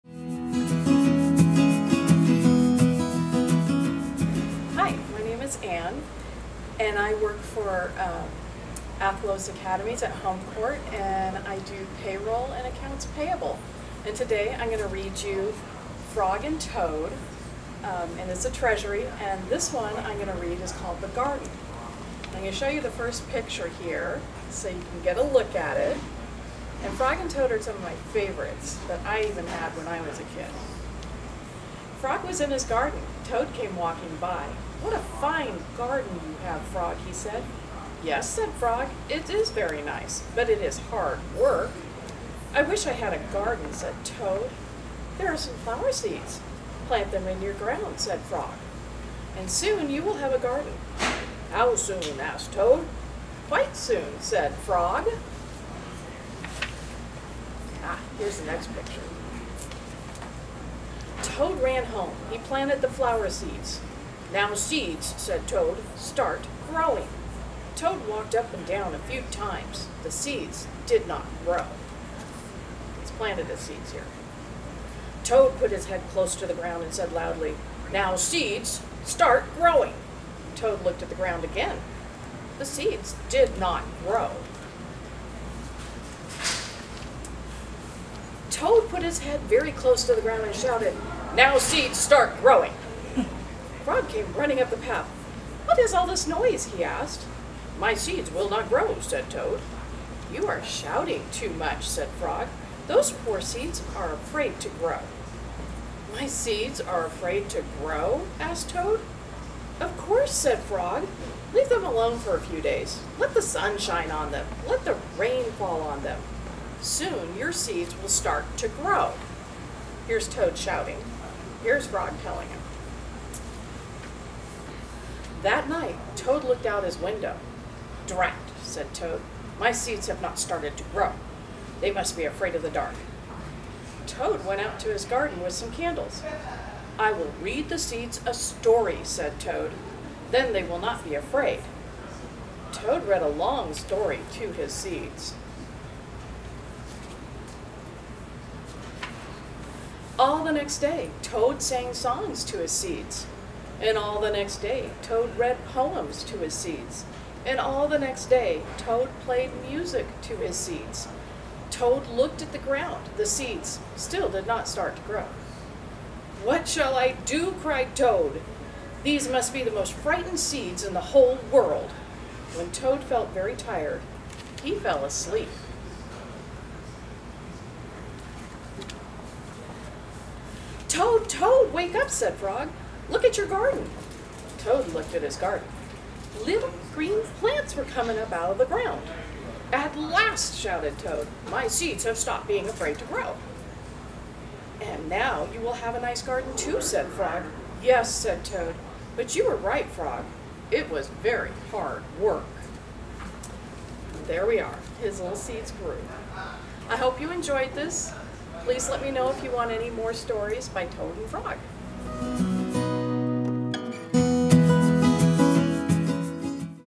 Tuck-in-Tuesdays - The Garden
Tuck-in-Tuesdays are back! 🌙 Tonight's story will be read by an Athlos HOmecourt team member! Anne Mraz reads, "The Garden" from The Frog and Toad Treasury by Arnold Lobel. 🐸